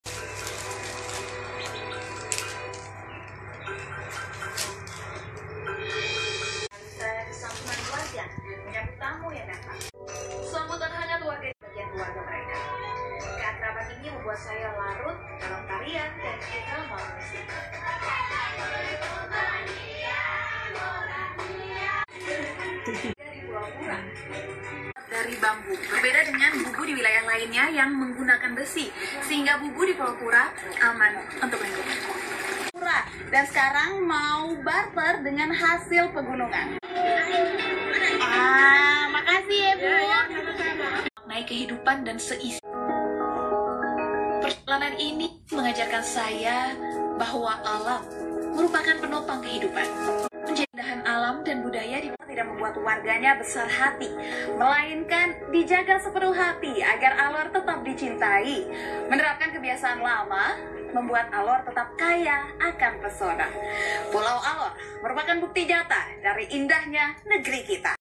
Shafinaz Nachiar
Shafinaz Nachiar ialah model cantik dan news anchor RCTI nan rupawan dengan senyuman manis serta tatapan mata sangat menawan hati.
Kunjungi laman kami di dan simaklah beragam video Shafinaz Nachiar.